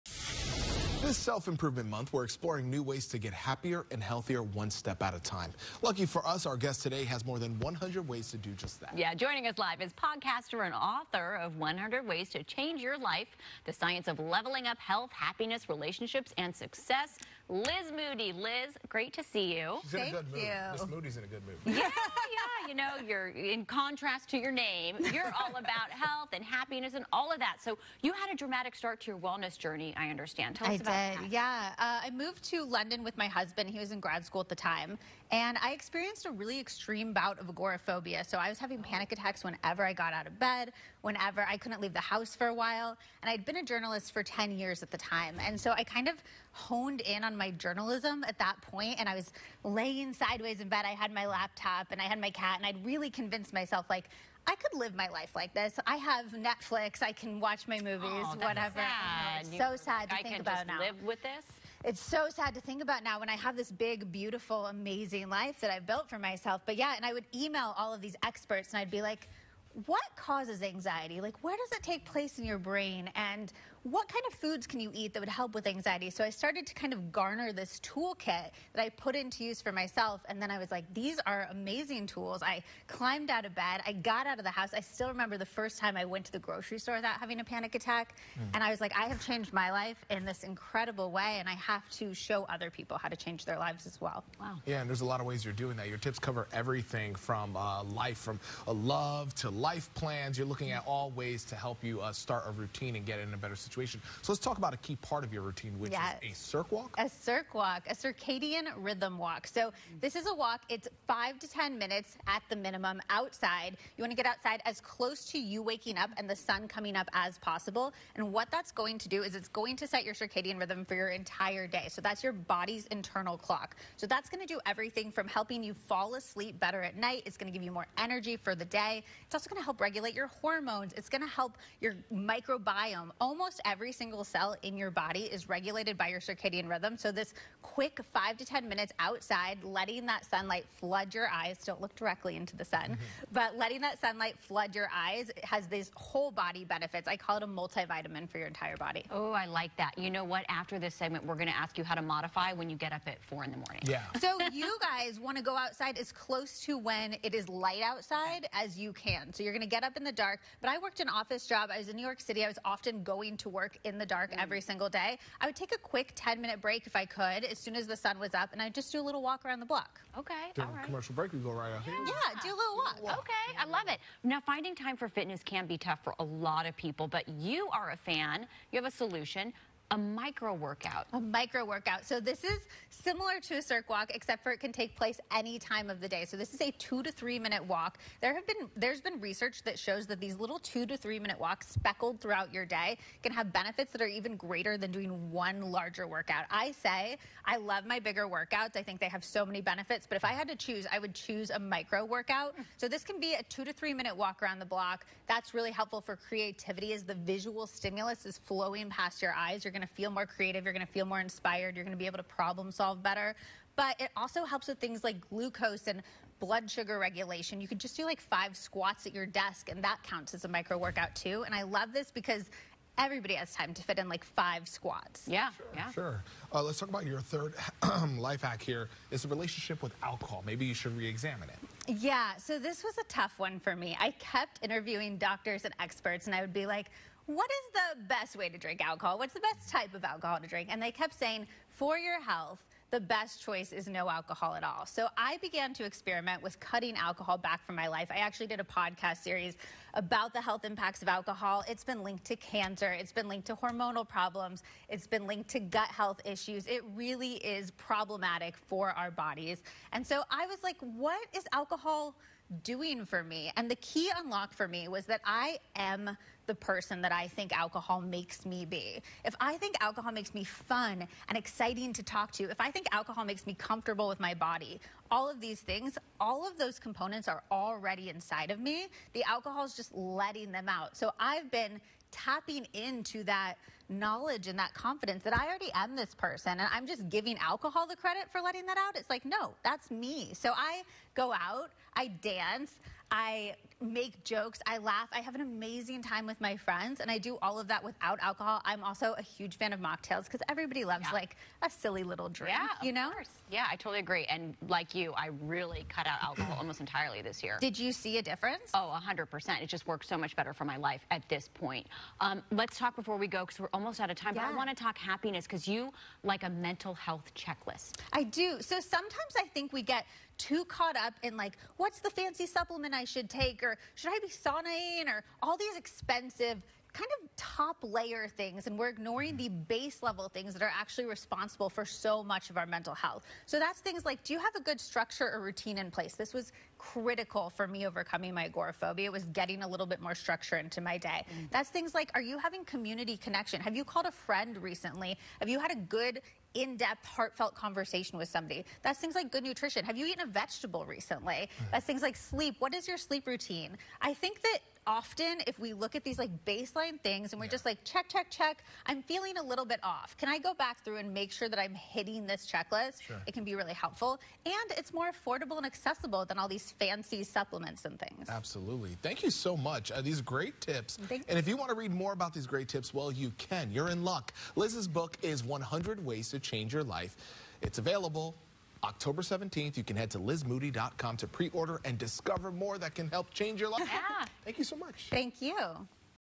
Liz Moody hones 100 Ways to Change Your Life
Liz Moody, podcaster and author of the upcoming book, 100 Ways to Change Your Life, offers life improvement tips in the way we like it today: small bites backed by science. She joined KTLA to discuss her latest venture, which she calls a "holistic guide to living your best life." 100 Ways to Change Your Life is out Oct. 17.  Aired Sept. 9, 2023.